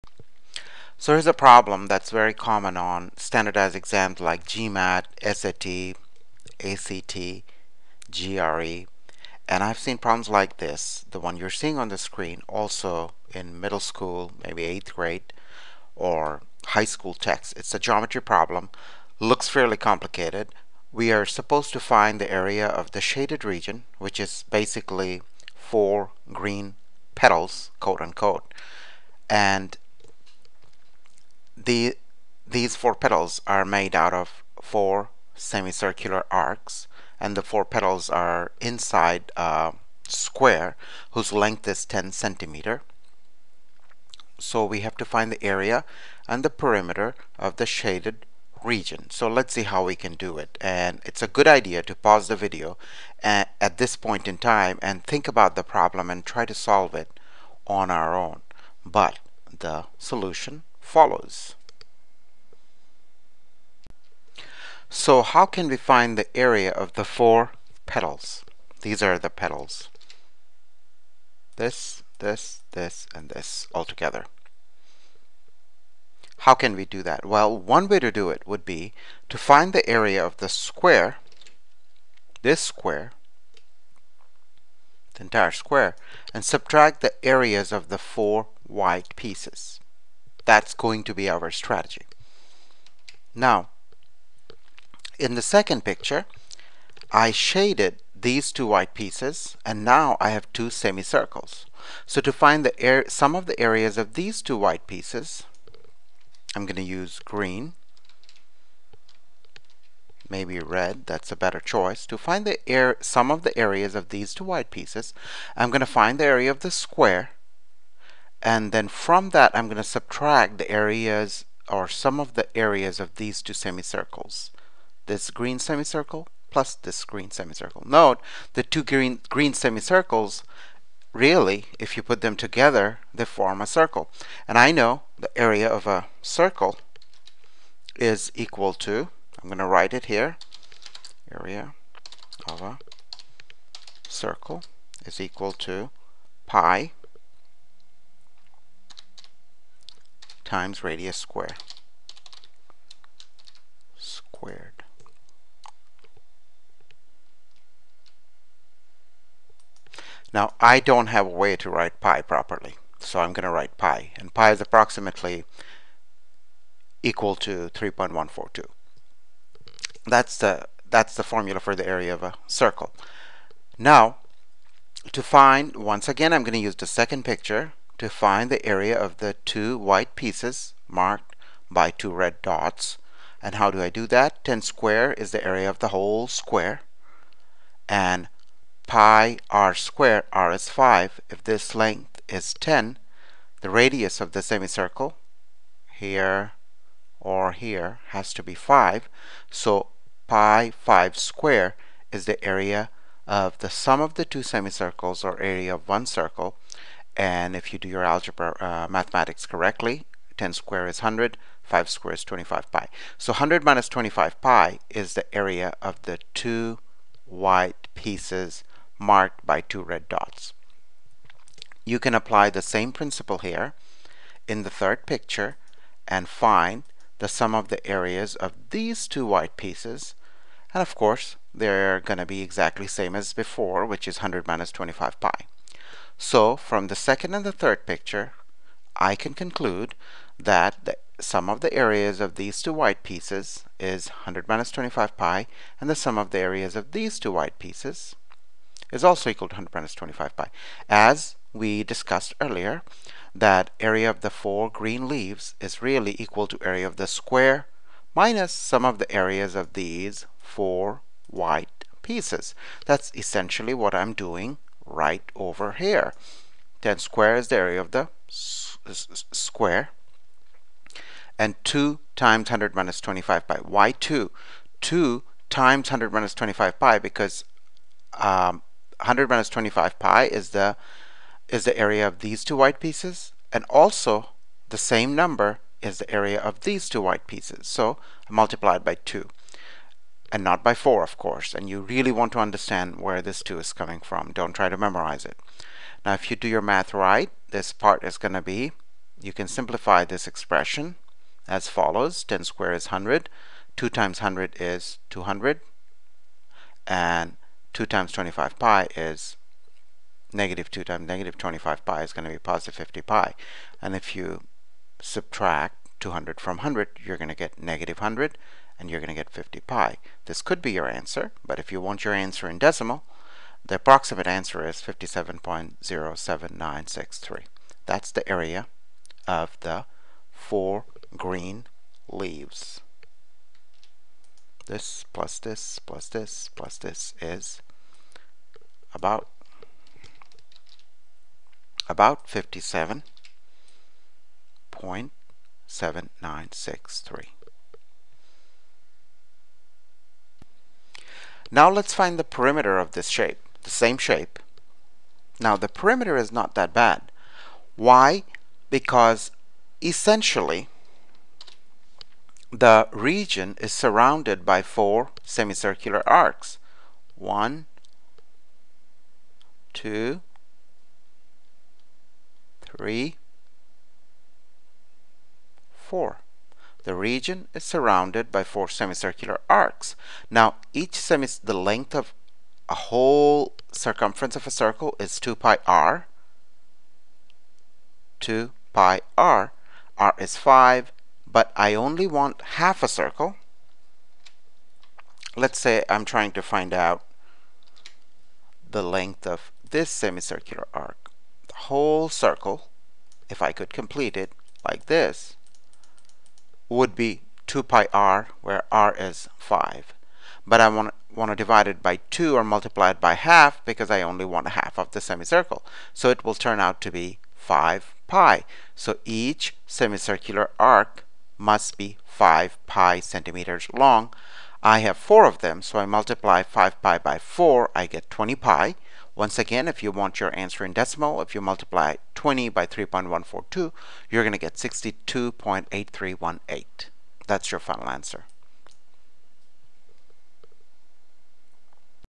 Area of Four Petals
This is a challenging geometry problem common in many standardadized exams like GRE, SAT, ACT, and GMAT